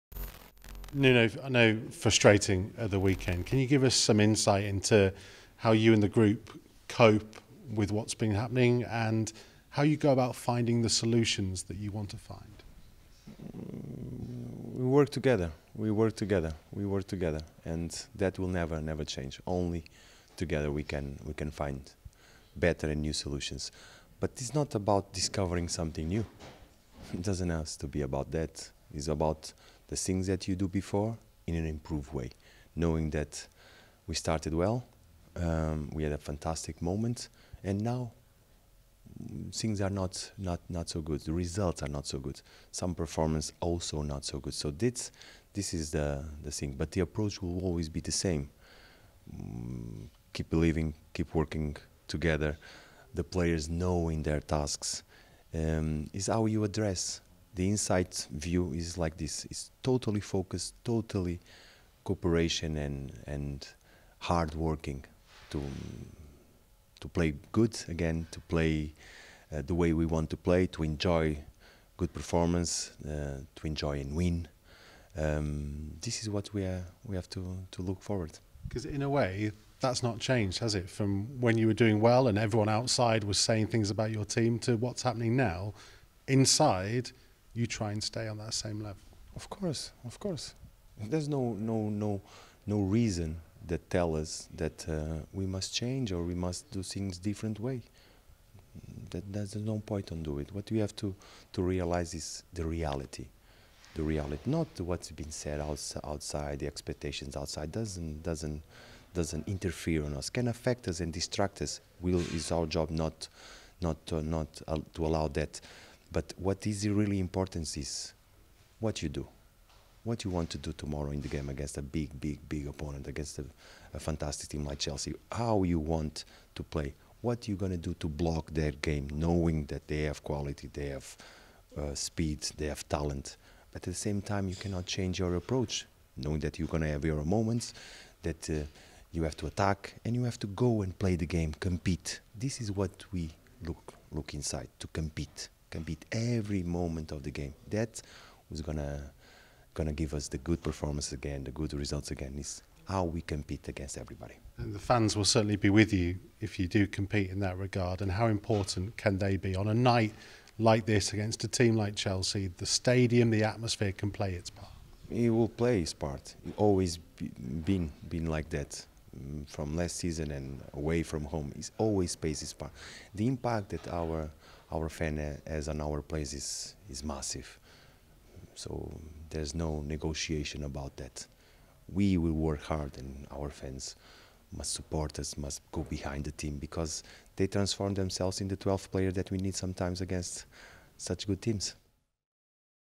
Nuno previews the match with Chelsea
Nuno speaks to Wolves TV ahead of the Premier League fixture with Chelsea at Molineux.